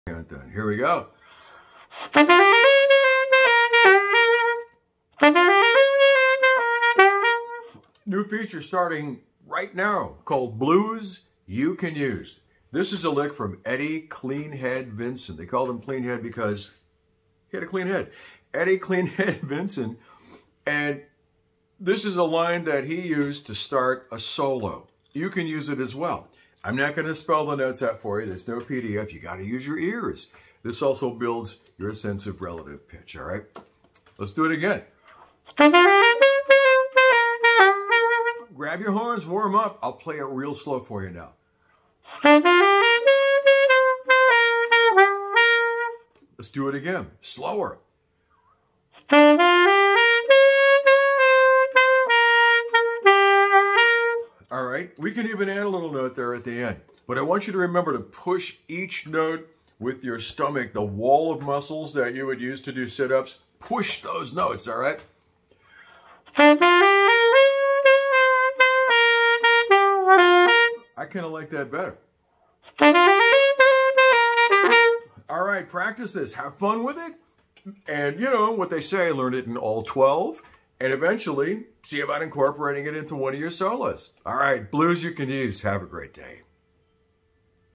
Blues You Can Use | Eddie 'Cleanhead' Vinson Sax Lick | Sax Lesson | Dave Good Sax
Friends, I'm starting a new feature called Blues You Can Use. Each week (I WILL try) to post a killer blues sax lick on this channel I've transcribed from a blues sax or guitar player. Your job is to likewise transcribe it to your horn. I've provided no PDF, key, or any information other than what you can get with your ears.
Eddie "Cleanhead" Vinson was a blues/R&B/jazz alto sax guy, and a prime singer too. Look him up on Youtube. His alto had a great, deep voice. I play tenor primarily, so I transposed his lick to my horn. Learn it, move it around on your horns, and try jump starting a solo with it.